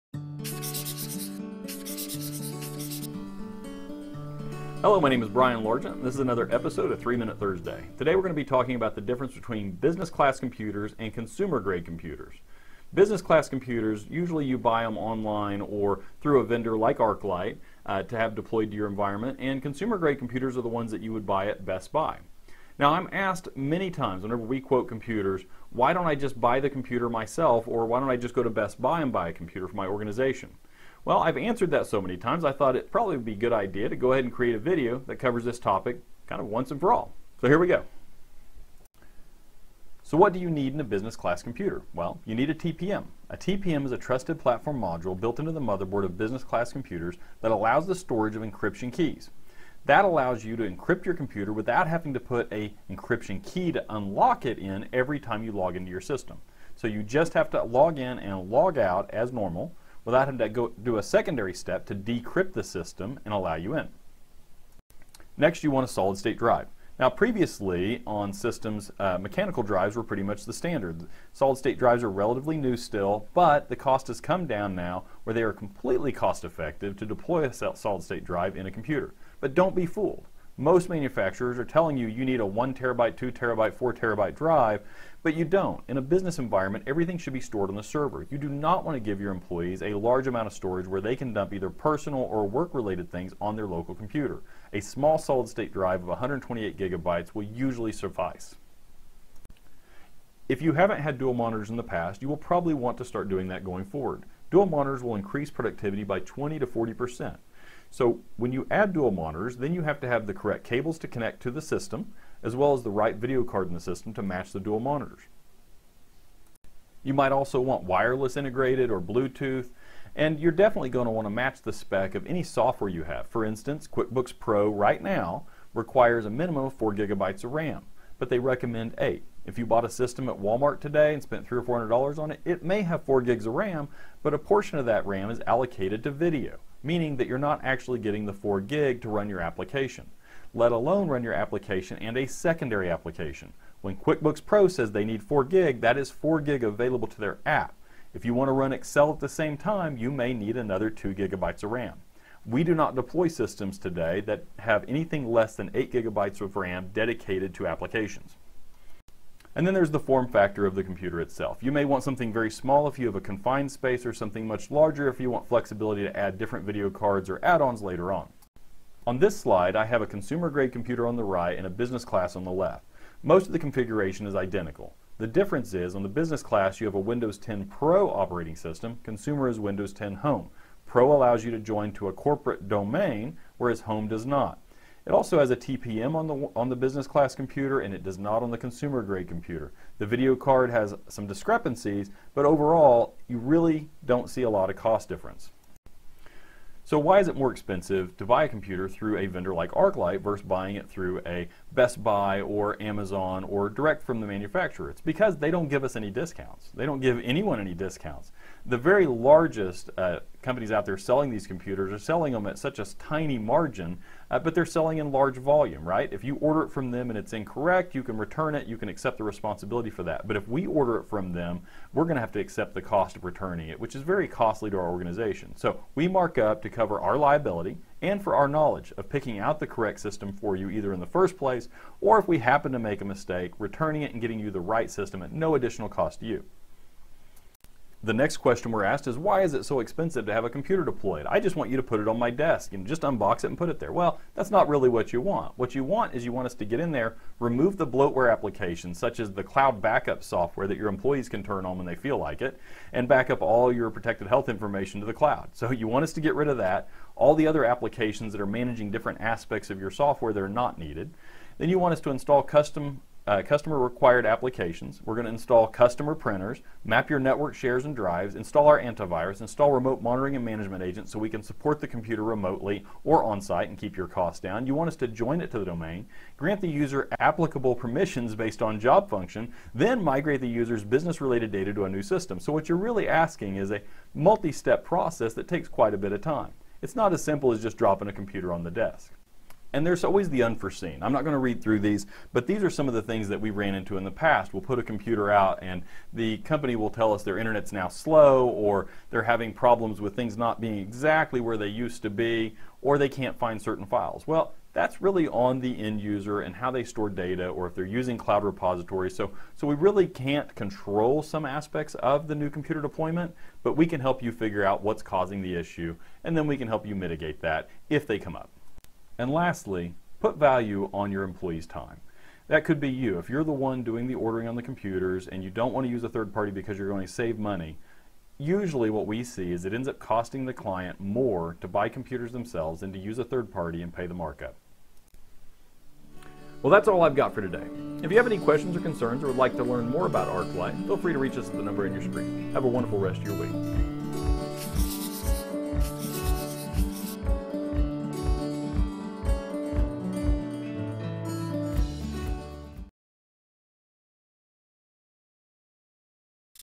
TMT 0018 Business vs Consumer Computers
What is the difference between business and consumer class computers?  Watch this brief video to learn about the differences as well as why you should utilize a third party like ArcLight to assist in picking the right system for your organization.